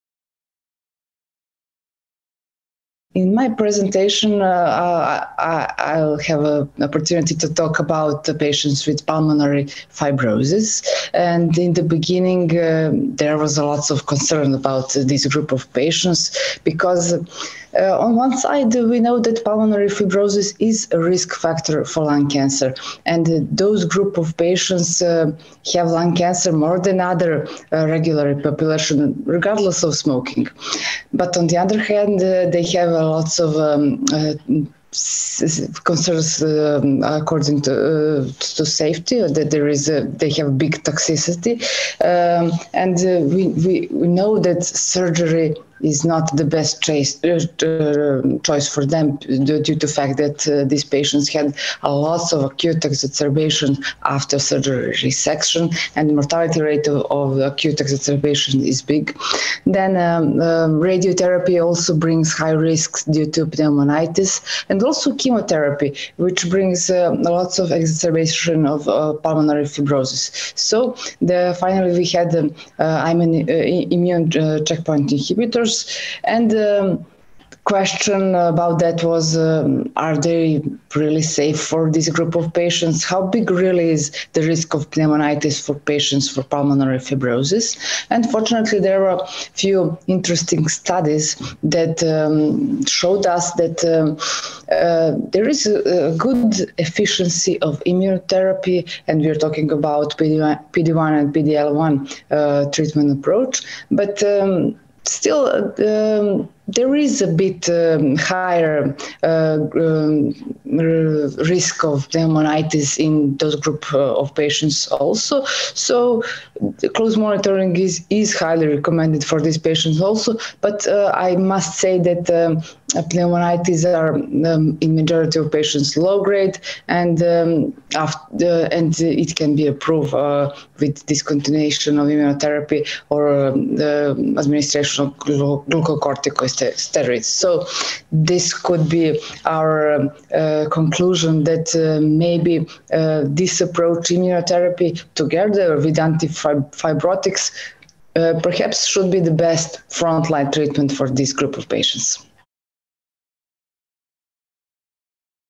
What is the risk of pneumonitis in patients with pulmonary fibrosis treated with ICIs?
Milica Kontic Jovanovic, MD, PhD, Clinical Center of Serbia, Belgrade, Serbia, discusses how immune checkpoint inhibitors (ICIs) carries a lower risk of pneumonitis in patients with pulmonary fibrosis compared to surgery, radiotherapy, or chemotherapy approaches. Dr Jovanovic continues by advocating that the most optimal line of therapy may consist of immunotherapy in combination with anti-fibrotic glucocorticoids. This interview took place at the European Lung Cancer Congress 2023 in Copenhagen, Denmark.